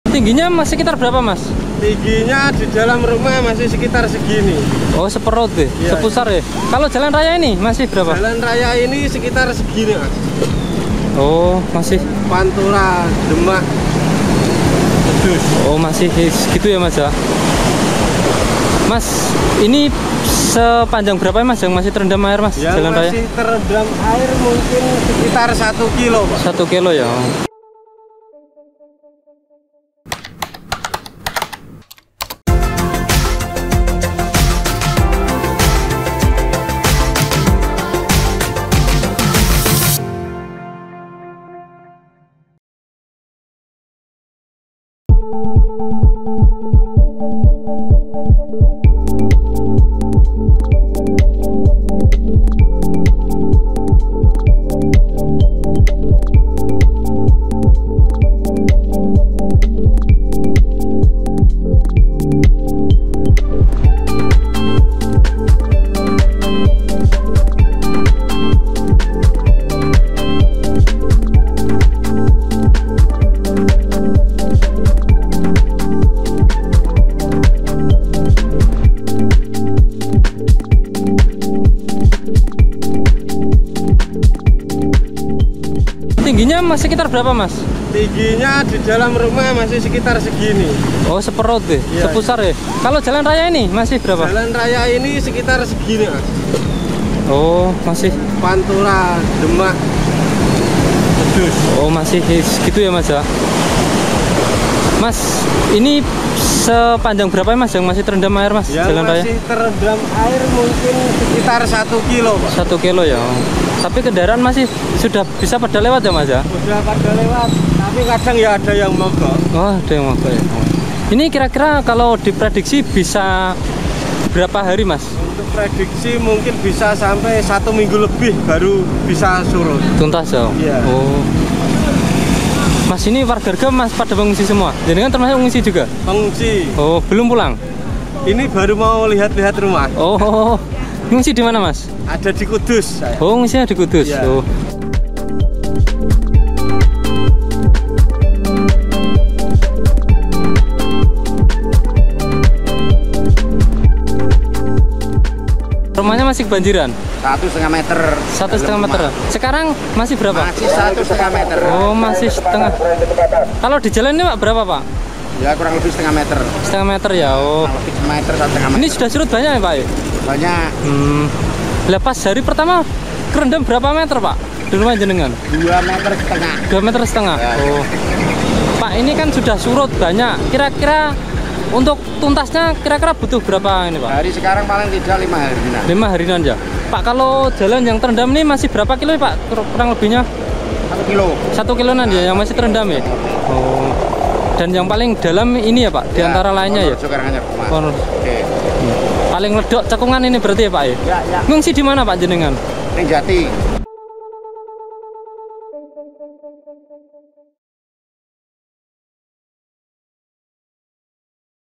Update Banjir Demak, Jalan Pantura yang Terendam Tinggal 1 Km
BETANEWS.ID, DEMAK – Banjir besar di Kecamatan Karanganyar, Kabupaten Demak yang merendam ribuan rumah dan melumpuhkan Jalan Pantura Demak-Kudus sudah surut. Saat ini jalan yang terendam banjir tinggal satu kilometer dengan ketinggian 35 centimeter atau sebetis orang dewasa.

Terlihat Jalan Pantura Demak yang masih terendam di sepanjang Desa Wonorejo, Kecamatan Karanganyar. Kendaraan roda dua maupun empat pun terlihat sudah lalu lalang melintas.